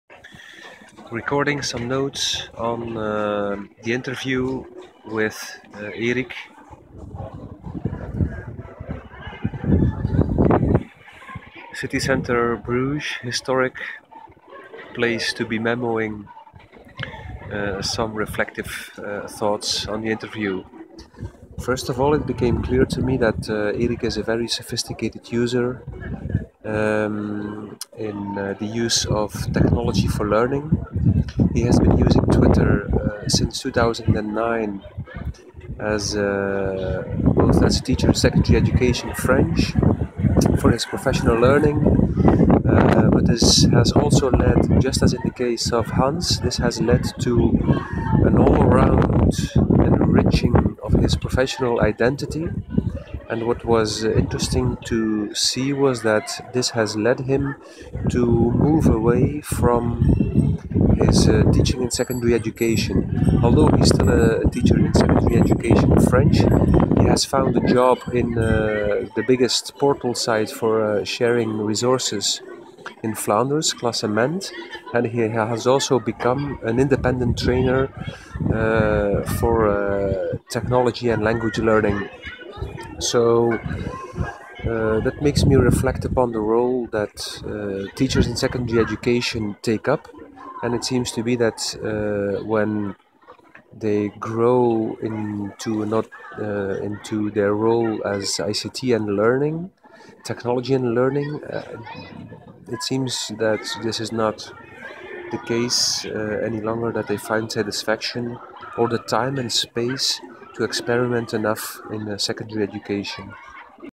First impression interview Erik 23/6/15
High level of technology usage. Self-directed learner. Observation: context of secondary education not very stimulating any more for Erik? E. combines three jobs: teacher French, ICT expert at klascement.be and independent trainer in TEL language teaching. The latter satisfy him more. Roles teacher ou-of-date??